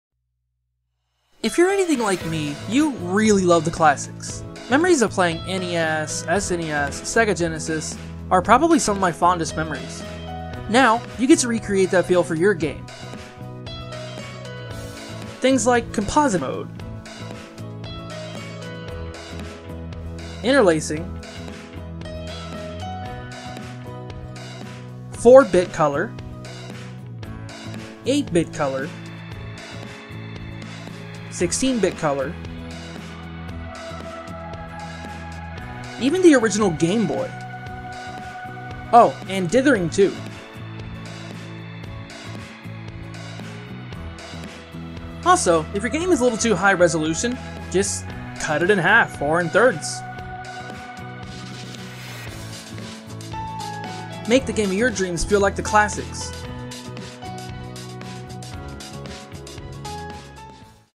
[MV] SuperRetro+! The Ultimate Retro MV Plugin
Finally, after working on this all day yesterday and today, it's done! The ultimate retro RPG Maker MV plugin. With these filters, you have the ability to enable composite mode, retro colors (4-bit, 8-bit, 16-bit, 24-bit), dithering, gameboy-mode, interlacing and more!

Given the amount of frustration I went through learning Shaders, and the fact I'm still jobless thanks to the covid situation, I unfortunately need to sell this product. However, I'm giving it to anyone who joins the channel if you wanted to do that instead of paying the full price.